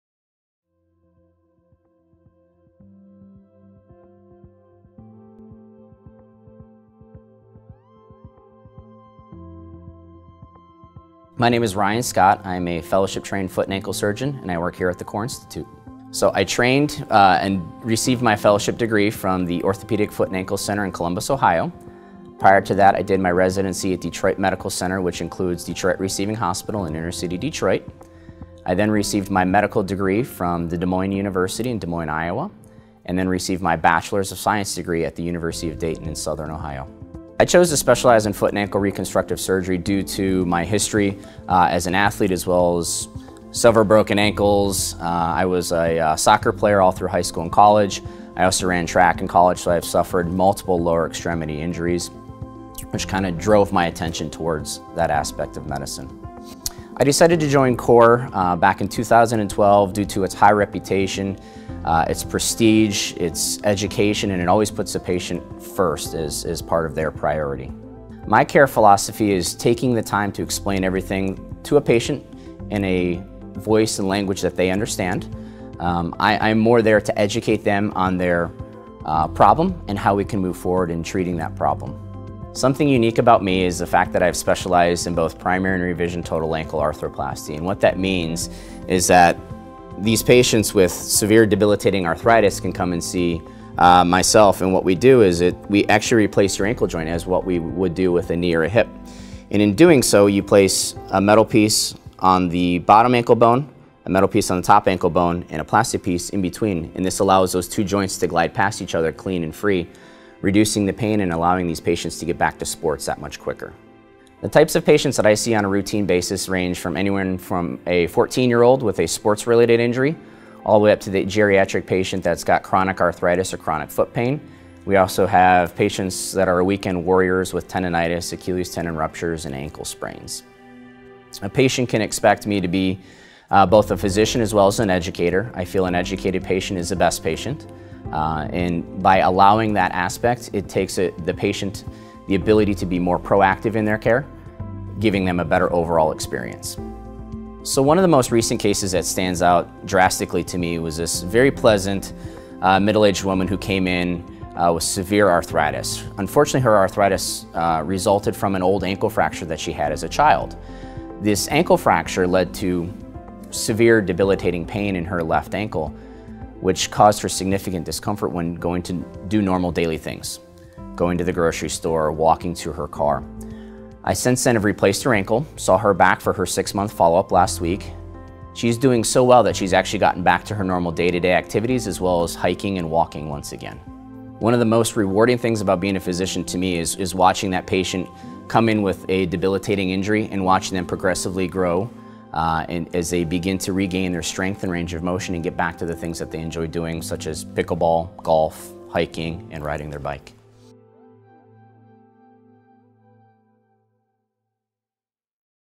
Get to the CORE of Dr. Ryan Scott, Foot and Ankle Surgeon
Ryan T. Scott, DPM is a fellowship-trained foot and ankle surgeon, specializing in primary and revision total ankle replacement, extensive reconstruction of the forefoot and hindfoot, sports medicine, arthroscopy of the foot and ankle, circular frames, and trauma. He is certified on INBONE, S.T.A.R., and Salto Talaris total ankle replacements. Dr. Scott is board certified in both foot surgery and hindfoot/ankle surgery.

Dr. Scott completed his fellowship at the Orthopedic Foot and Ankle Center in Ohio, where he received comprehensive orthopedic foot and ankle training. Prior to his fellowship, he completed his podiatric surgical residency at the Detroit Medical Center in Michigan, where he functioned as Chief Resident. During his residency, Dr. Scott received multi-disciplinary training in orthopedic surgery, general surgery, internal medicine, vascular surgery, reconstructive plastic surgery, infectious disease, and diabetic limb salvage.

Dr. Scott received his medical degree from Des Moines University. There, he served as Vice President of the American College of Foot and Ankle Orthopedics and Medicine student chapter, was a student mentor for the American Association of Colleges of Podiatric Medicine, and represented his college as a student ambassador. Dr. Scott obtained his Bachelors of Science degree in Exercise Physiology from the University of Dayton in Ohio. He has volunteered for the Michigan Special Olympics Fit Feet, where he provided medical screenings for the Special Olympic athletes.

Additionally, Dr. Scott has helped advance the profession through numerous publications and nationally attended lectures within the field of foot and ankle surgery. Dr. Scott is also an active member of the American College of Foot and Ankle Surgeons (ACFAS) and lectures and teaches other colleagues within the field while serving on peer appointed committees.